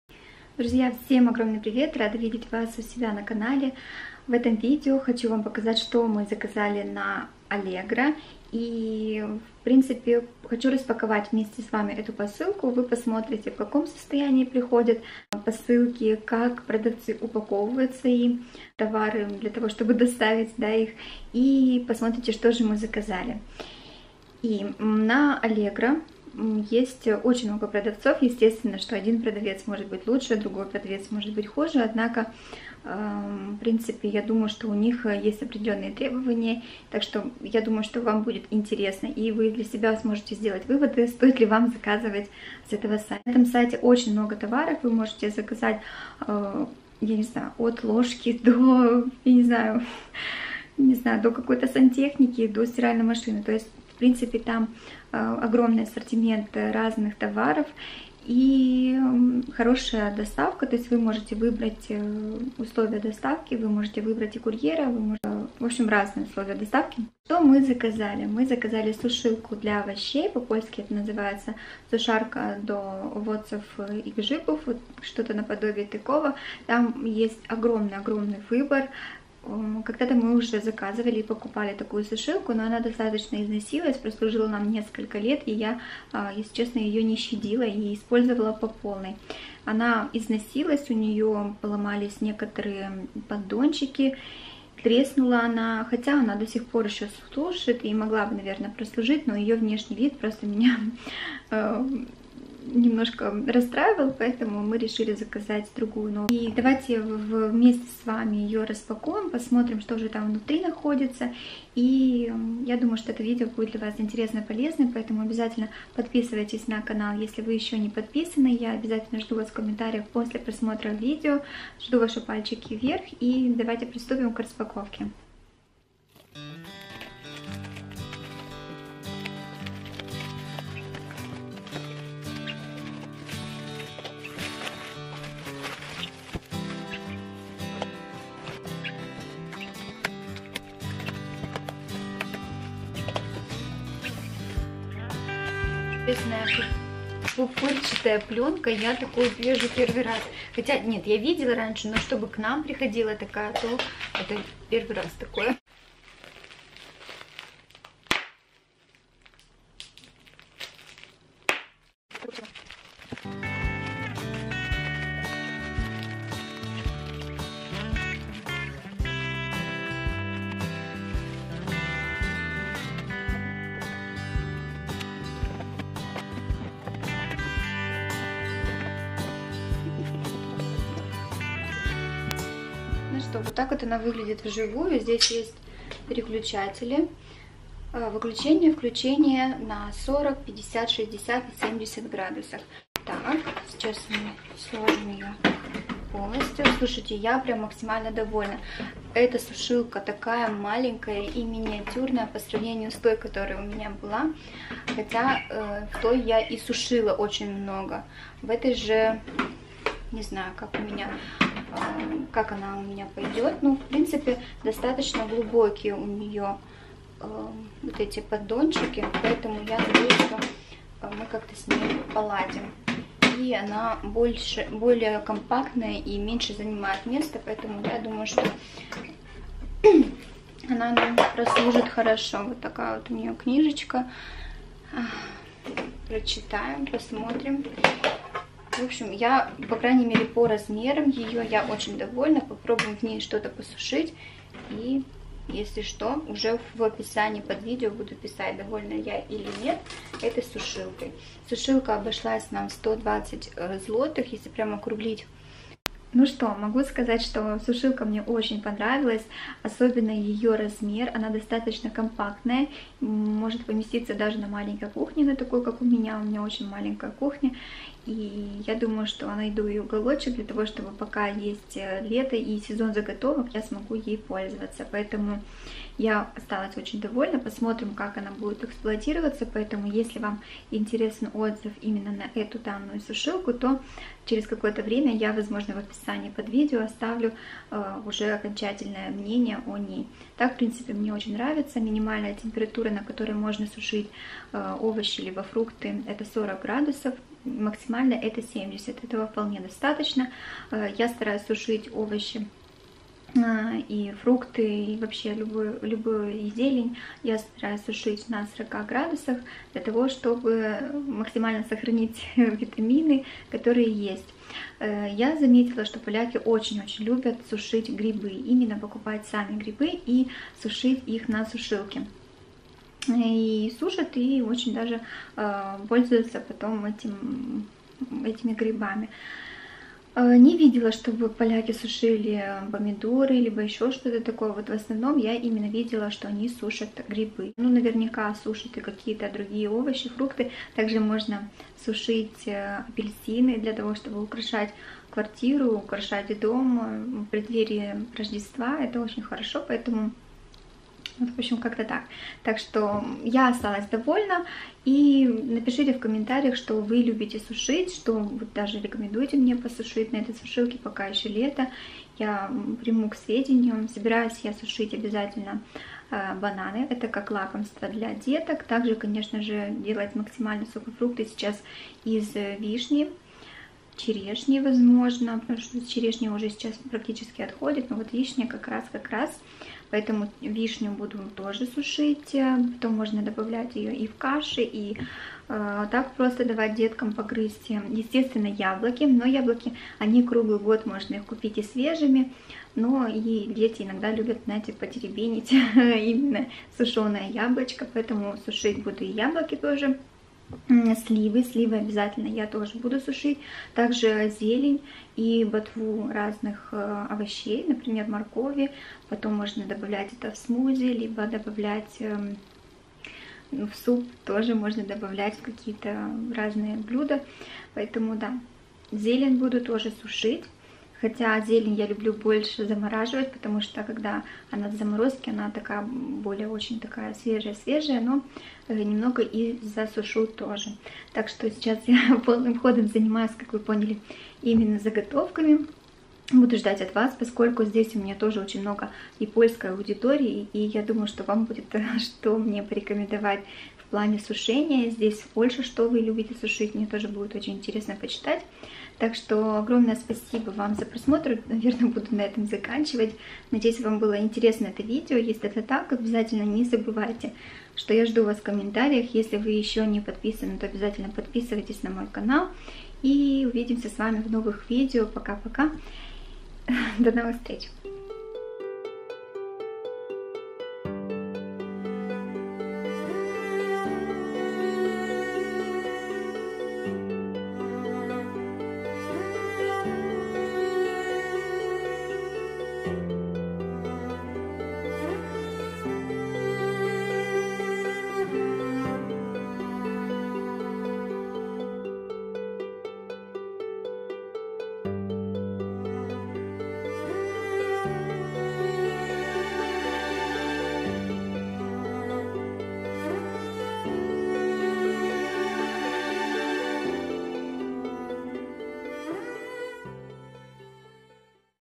ПОКУПКИ В ПОЛЬШЕ качество товаров в Польше, allegro
Друзья всем привет! Сегодня я покажу вам что входит в обычную жизнь в Польше, конечно же покупки, покупки на польском сайте Allegro, где покупать в Польше. Жизнь в Польше 2019, работа в Польше, карта побыту в Польше, эмиграция в Польшу, адаптация в Польше.

Я всегда очень жду ваши комментарии!

1807